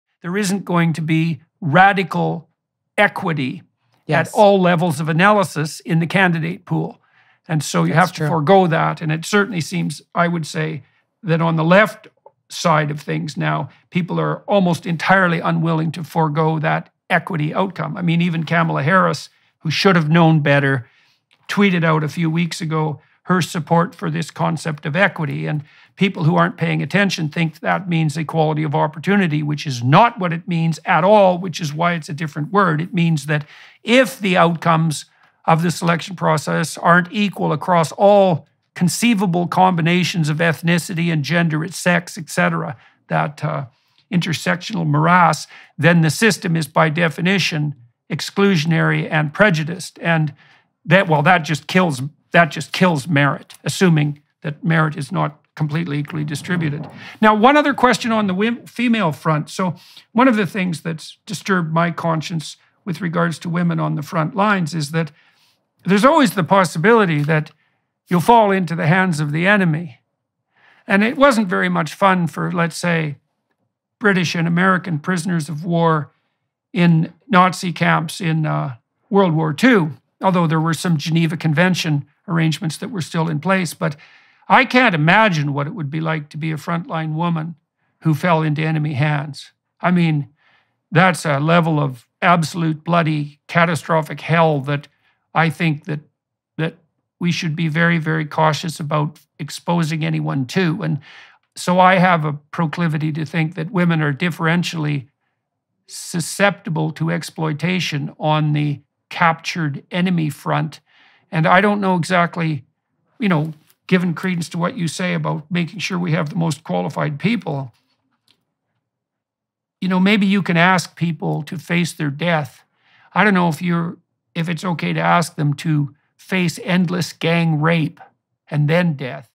JORDAN PETERSON: WOMEN ARE NOT EQUAL!
Women equality | Jordan Peterson

Jordan Peterson talks about women equality.